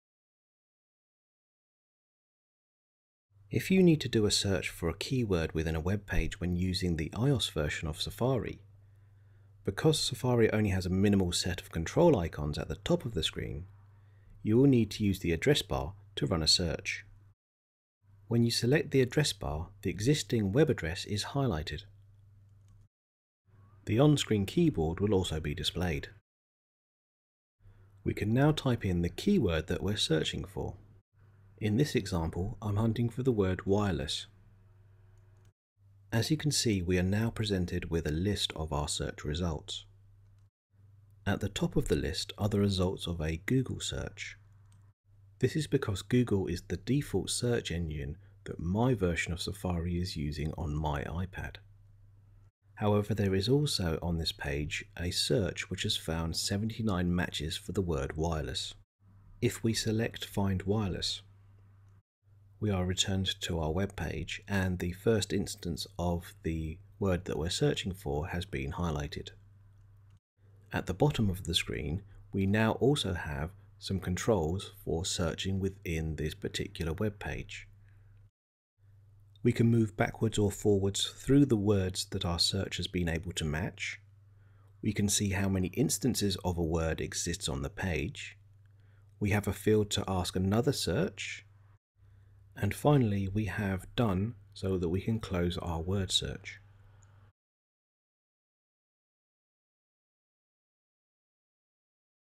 iOS tip - How to do a Keyword search with iOS Safari
In this video we will show you how you can search for keywords on a webpage from within the iOS version of Safari.